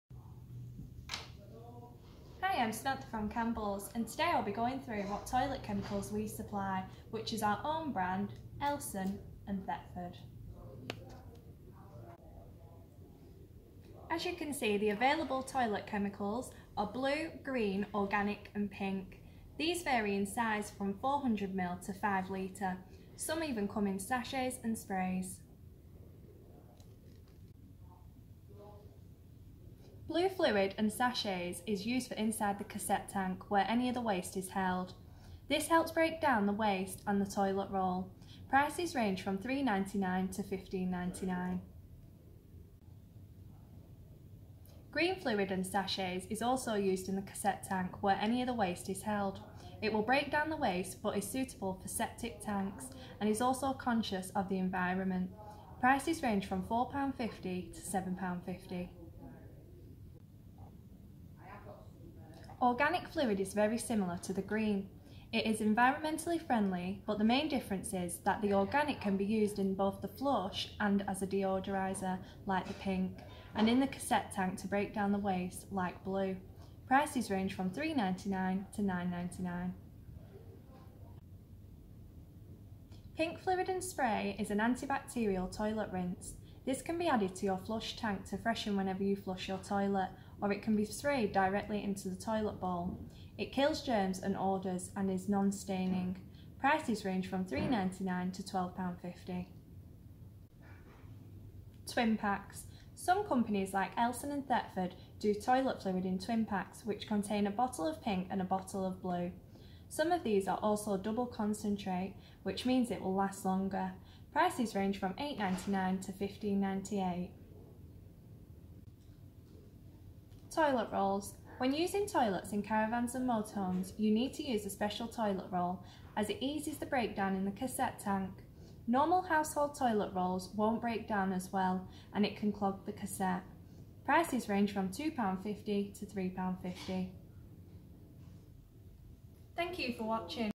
Beginner's Guide: Toilet Fluids
Welcome to Campbells. Here is a short video on toilet fluids, what we sell and what's next for you.